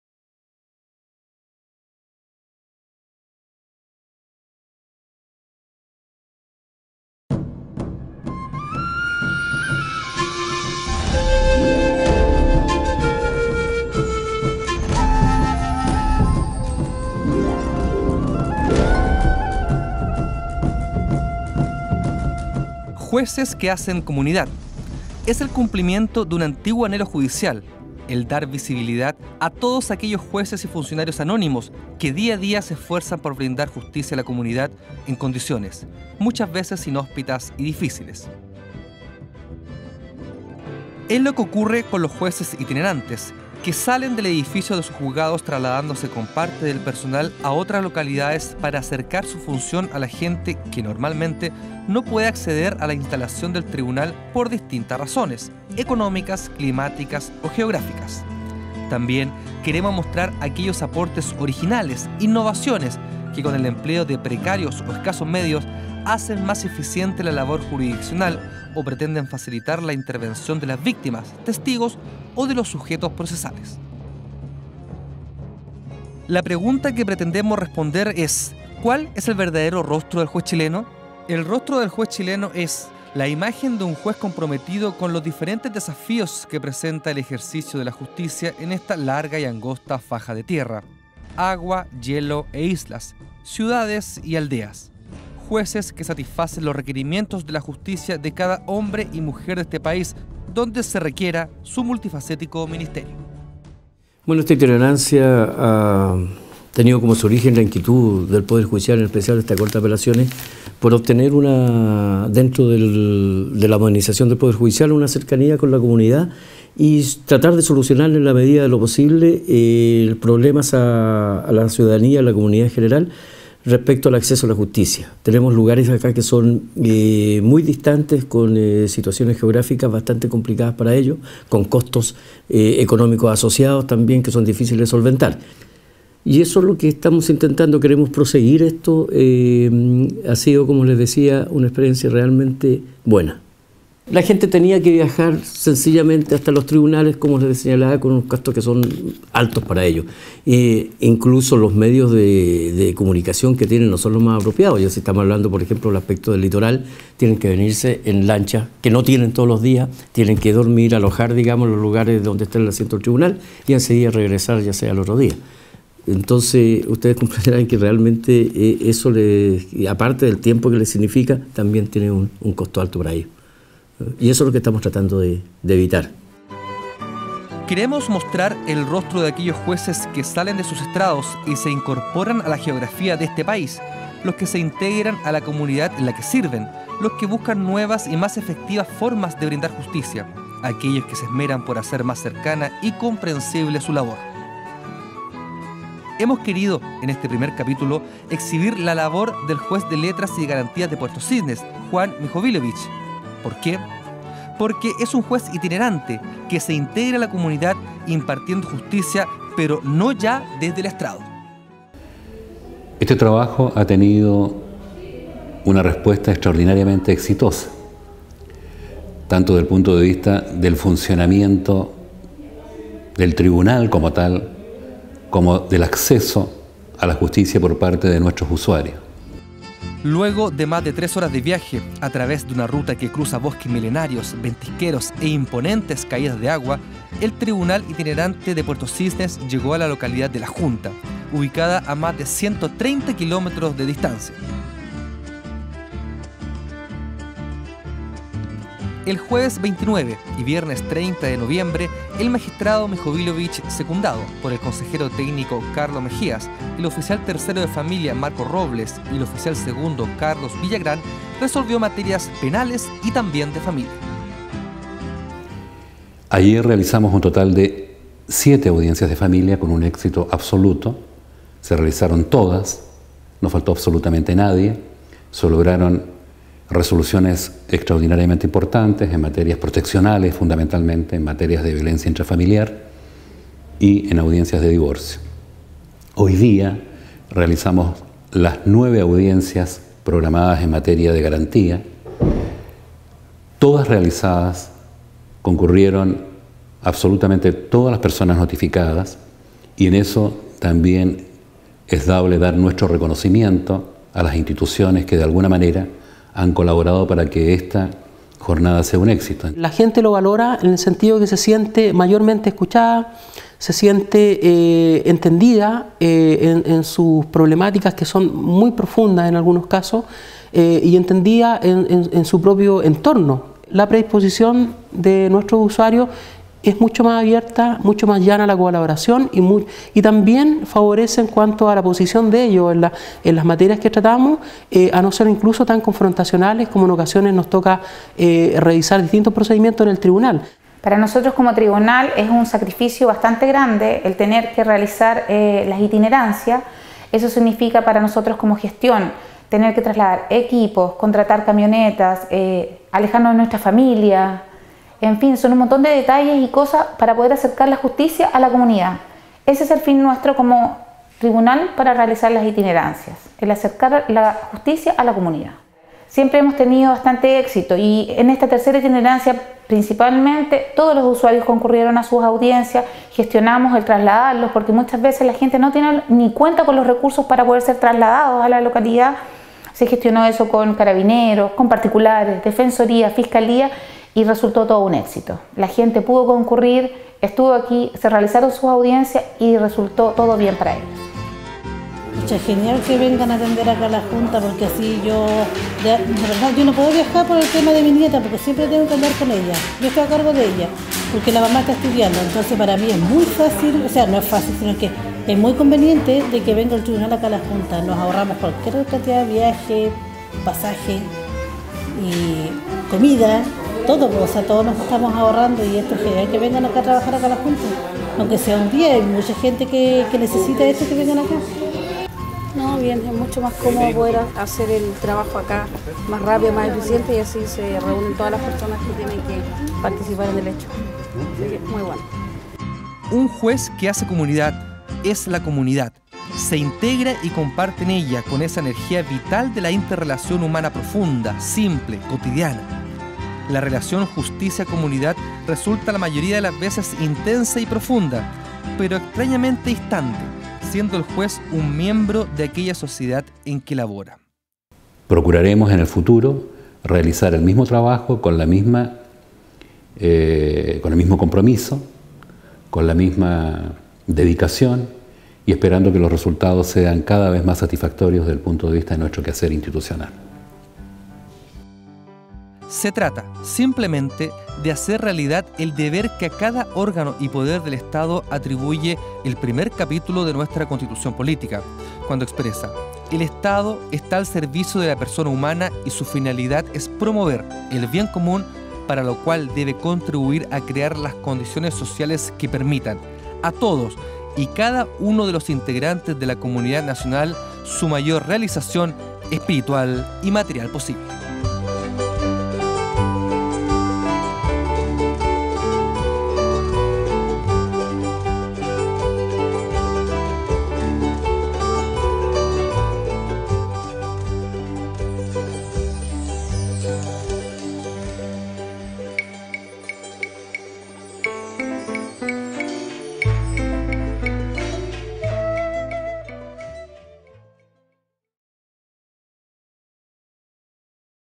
Jueces que hacen comunidad: Juzgado Mixto de Puerto Cisnes en la localidad de La Junta
"Jueces que hacen comunidad", es la primera entrega de un proyecto impulsado por el Poder Judicial que, a través de la producción de video-documentales, tiene por objetivos: permitir la visualización de los magistrados en su cotidianidad y acercar su trabajo a la comunidad, resaltar aportes innovadores y potenciar la misión de servicio público que realizan los funcionarios judiciales de todo el país.

En este primer capítulo, se aborda el tema de los tribunales itinerantes, por medio del seguimiento y registro de la constitución del Juzgado Mixto de Puerto Cisnes en la localidad de La Junta (XI Región).

En próximas entregas se tratarán temáticas, tales como: Salas Gesell y salas especiales de la justicia penal infanto-juvenil: Tribunales de Tratamiento de Drogas; Las visitas de jueces a hogares de menores, centros de internación y cárceles; Los juzgados rurales o en zonas geográficas extremas, y jueces o ministros con dedicación exclusiva, entre otras.